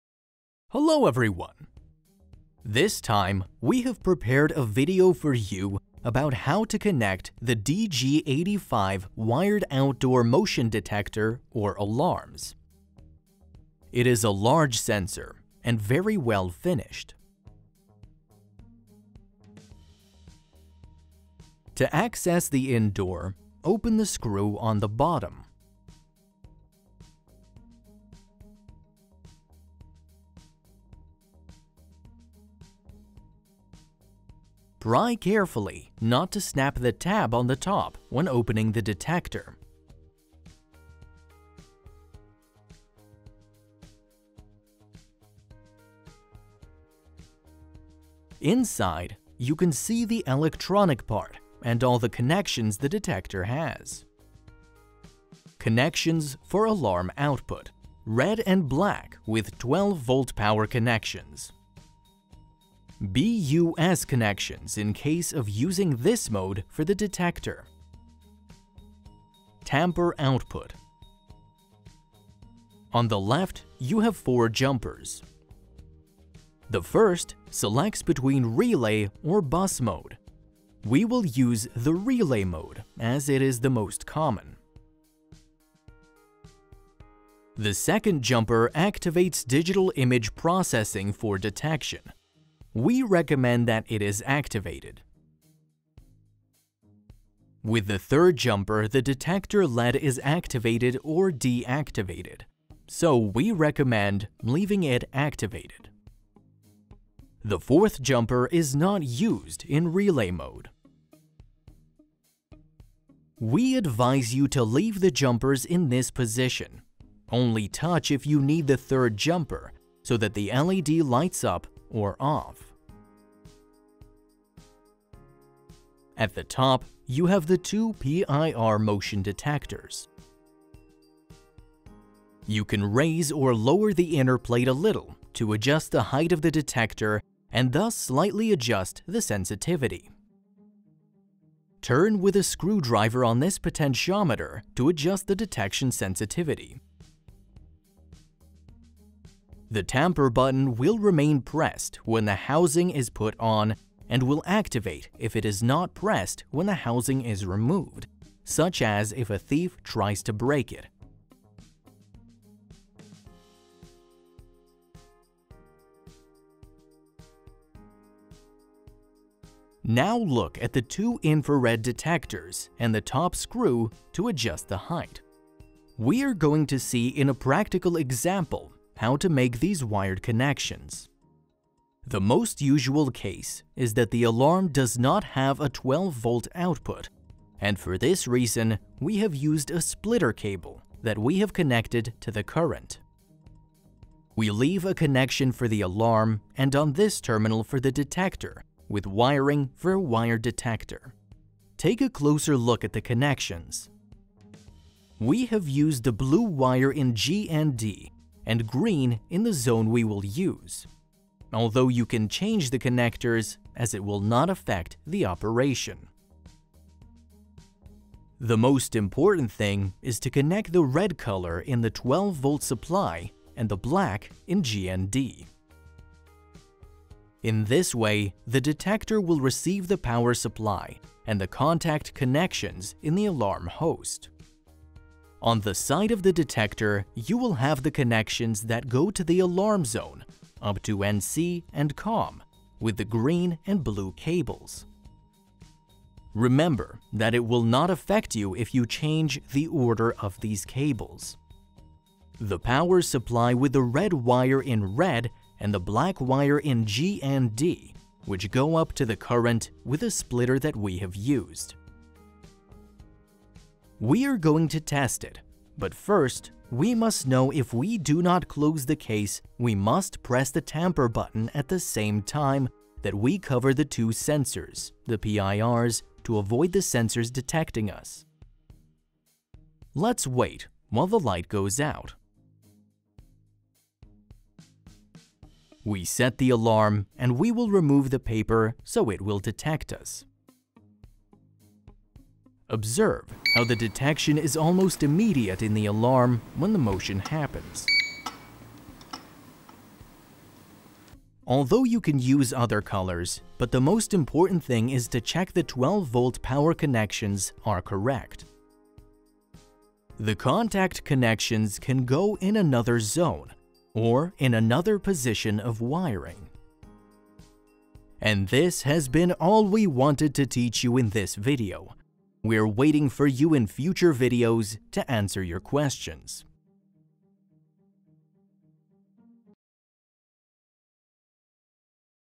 How to install wired sensor in alarm. Outdoor motion detector alarms with cable connection
In this video we will show you how to make the connections of a wired detector such as the DG85 model. We will show you how the detector is internally in its electronic part to explain all its connections and what each component is for.

At the end of the video we will show you a practical example on how to connect the detector to an AZ017 alarm, obtaining the power supply externally to an alarm center.